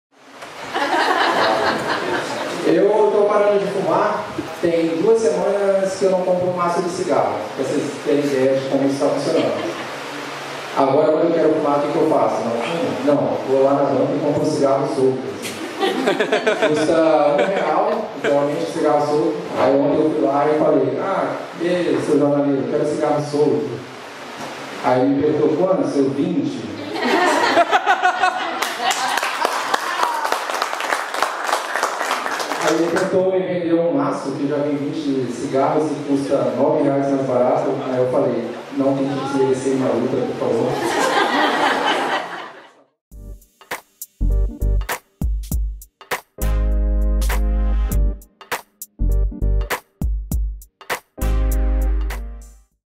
PARANDO DE FUMAR - Rafael Parreira - Stand-up Comedy
Rafael está parando de fumar para provar que ele consegue parar de fumar. Quando provar que consegue parar de fumar, ele volta a fumar, pois já provou que conseguia parar de fumar.

Gravado na noite de open mic do Capivara Comedy, em São Paulo - SP, dia 01/09/2019.